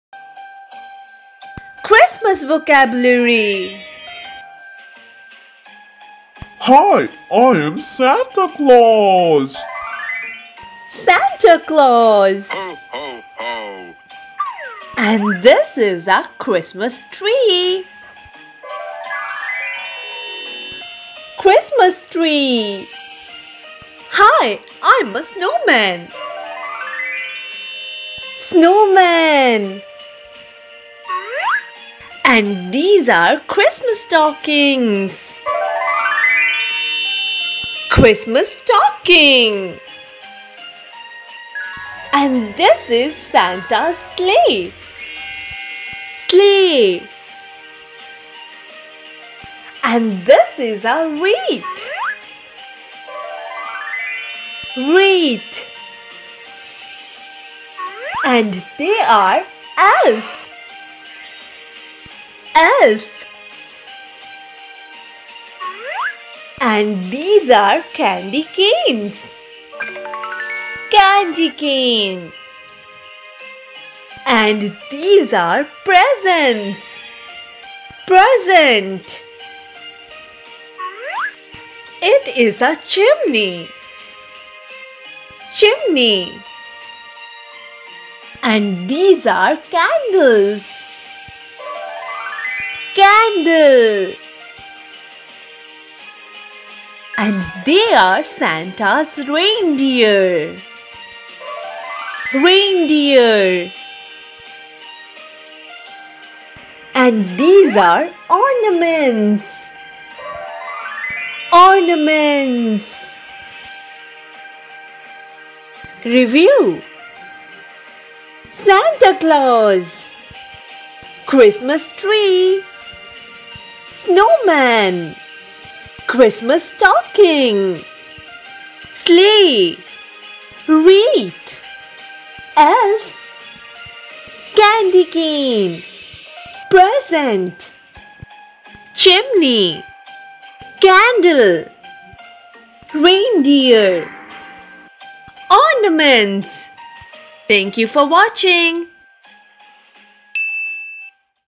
Christmas Vocabulary for Kids
Let's learn some Christmas Vocabulary in english with us this Christmas

Santa
Reindeer
Christmas tree
Snowman
Christmas Wreath
Christmas Stocking
Elf
Chimney
Ornaments
Candy cane
Candle